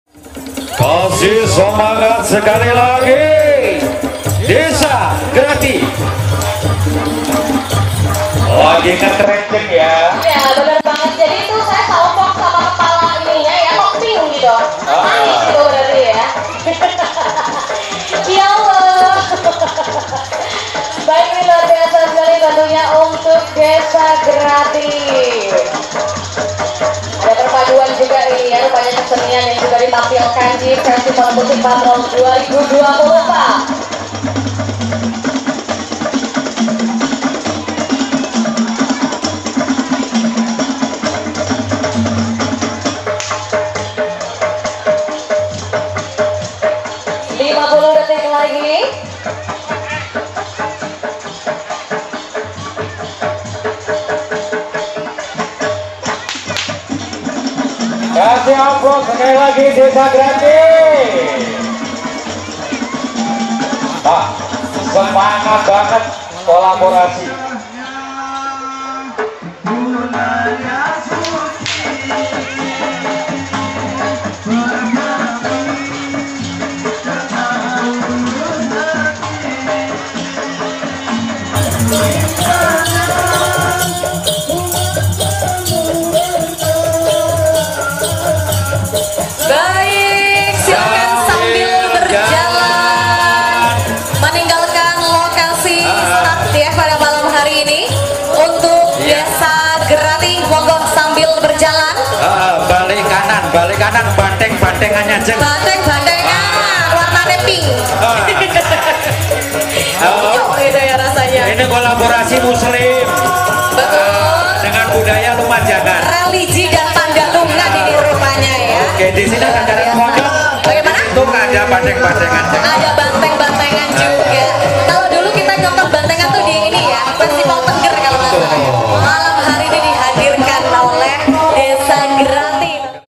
Peserta dari Desa Grati (Festival Musik Patrol 2024 Masjid Agung Lumajang)
Ini adalah penampilan no urut 5, Peserta dari Desa Grati di acara "Festival Musik Patrol 2024 Masjid Agung Lumajang".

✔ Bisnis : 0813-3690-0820